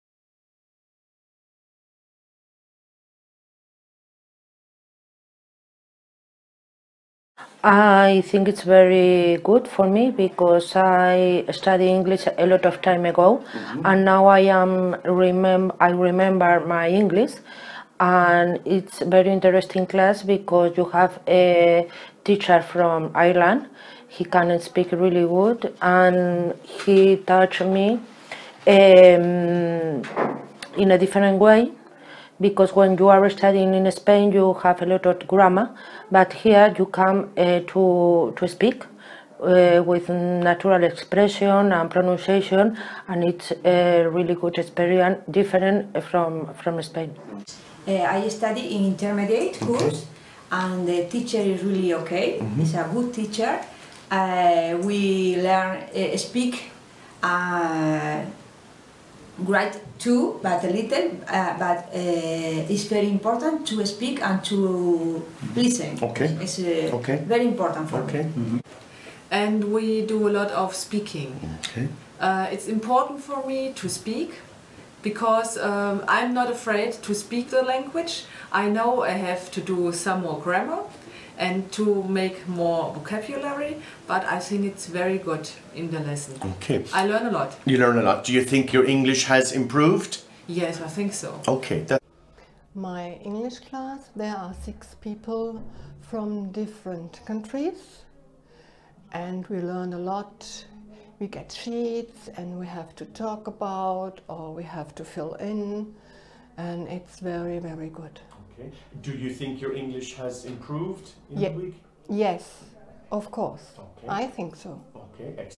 What do we study at Intermediate level
English language students (EFL) of Killarney School of English, Killarney Ireland discuss what they study at intermediate level (B1) in the General English classes.